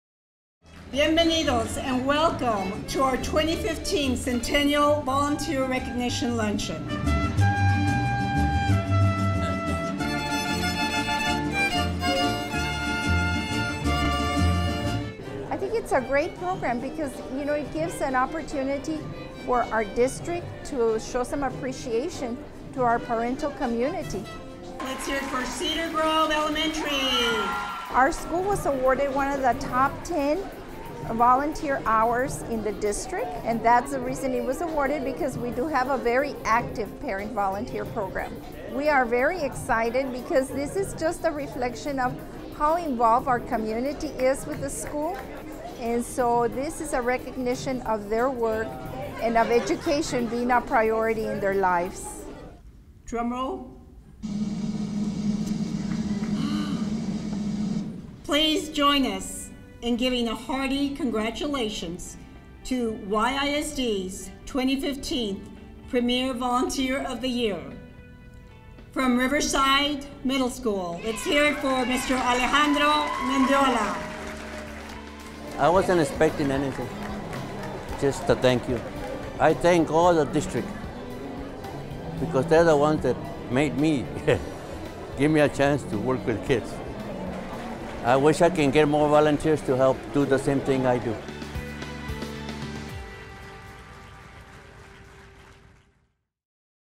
2015 Volunteer Luncheon
About 800 school volunteers and staff celebrated their hard work and dedication at YISD's annual Volunteer Recognition luncheon. The event honored their generosity and hard work at their individual campuses throughout the year.
By; Alexis Ortiz